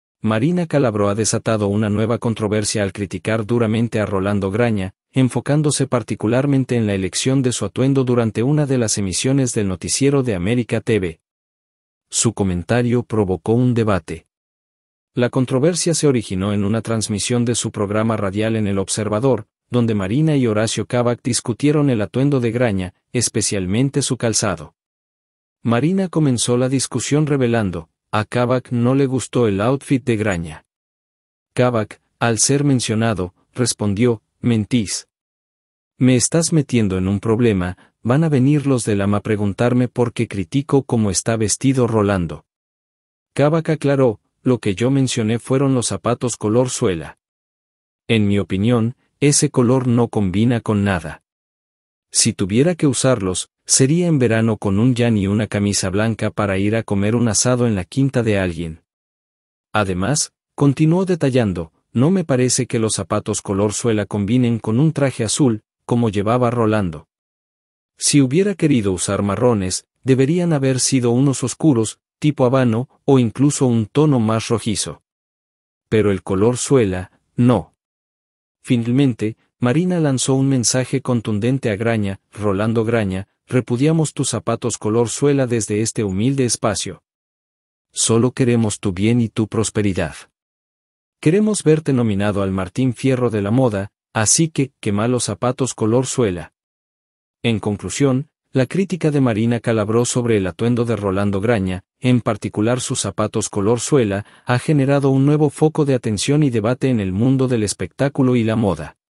La durísima crítica de Marina Calabró a Rolando Graña que desató un escándalo
La periodista lanzó una sorpresiva acusación contra el conductor del noticiero.